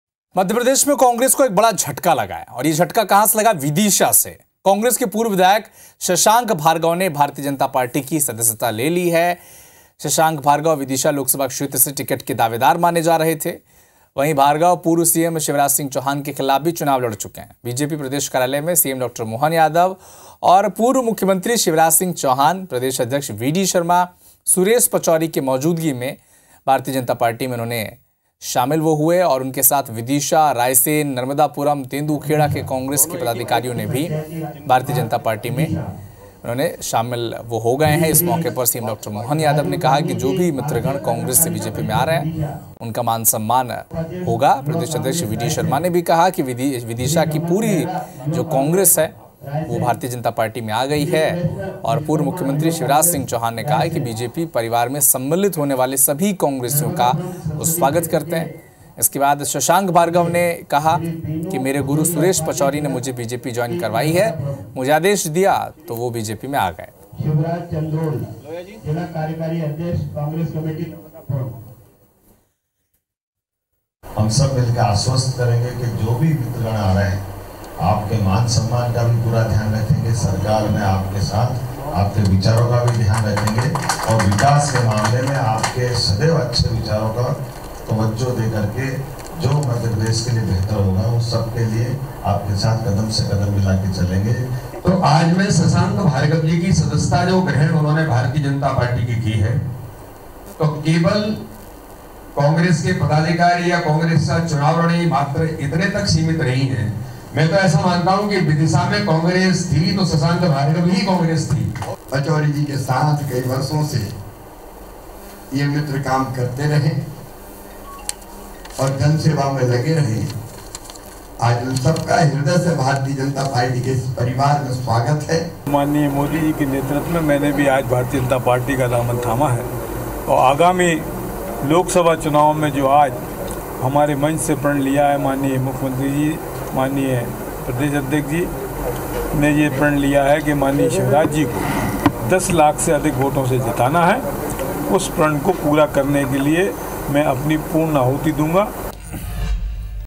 Shashank Bhargava Join BJP: MP Congress को बड़ा झटका | पूर्व विधायक शशांक भार्गव BJP में शामिल
IBC24, Shashank Bhargava Join BJP, MP Congress को बड़ा झटका, पूर्व विधायक शशांक भार्गव BJP में शामिल